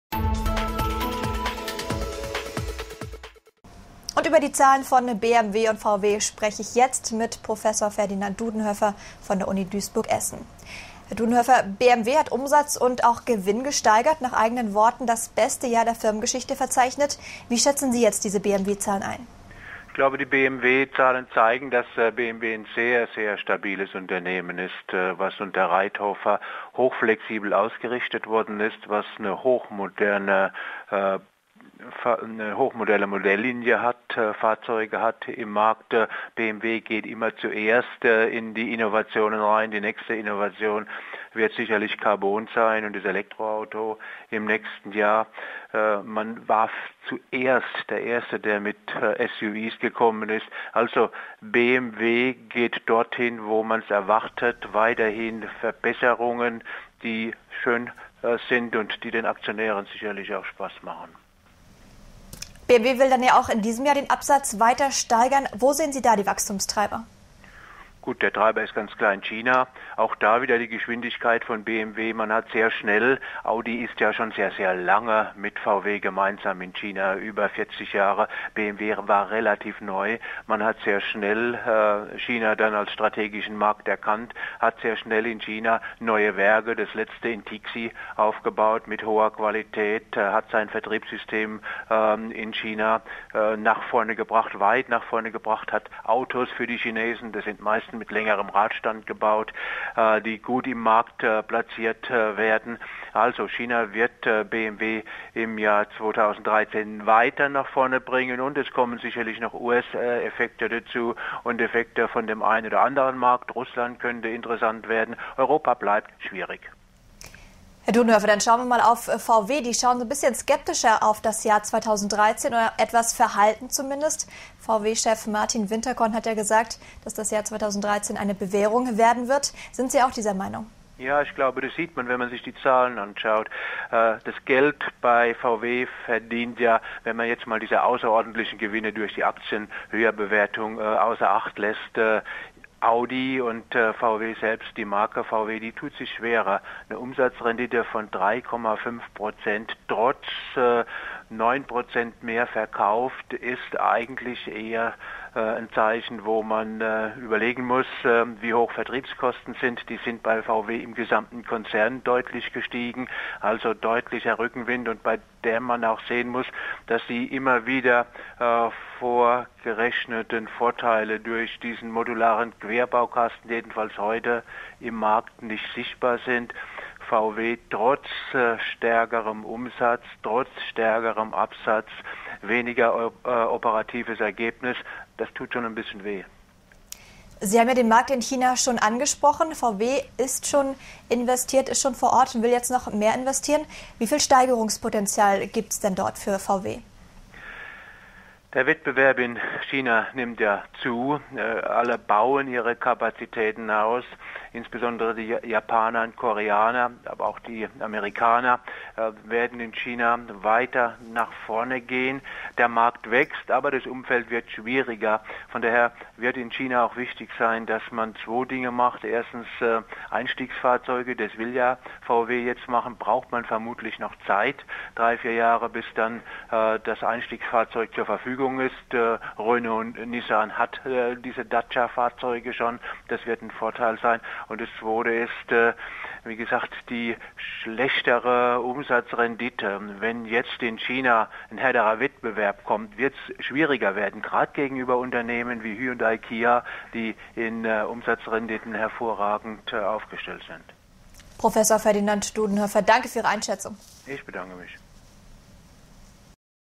BMW: China wird der Antreiber für 2013
BMW hat am Donnerstag Geschäftszahlen veröffentlicht und konnte Umsatz sowie Gewinn deutlich steigern. Nach eigenen Aussagen sei es das beste Jahr der Firmengeschichte überhaupt gewesen. Prof. Ferdinand Dudenhöffer von der Universität Duisburg-Essen bezieht im Interview Stellung zu den Zahlen.